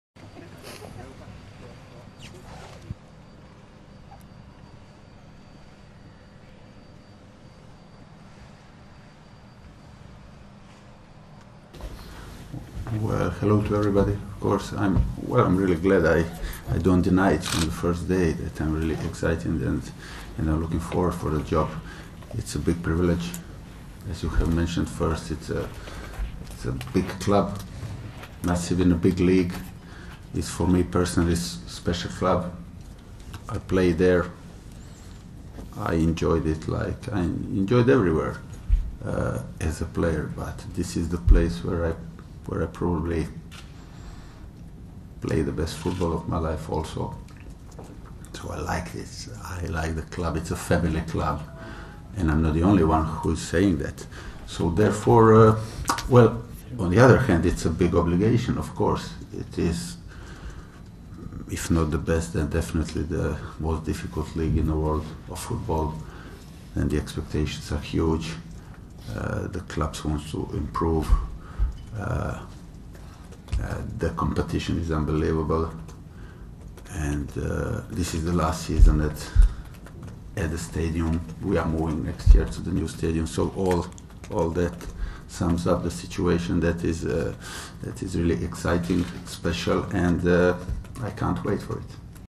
Slaven Bilic discusses why he chose to...
New manager Slaven Bilic discusses why he chose to come to West Ham and what challenges and expectations he faces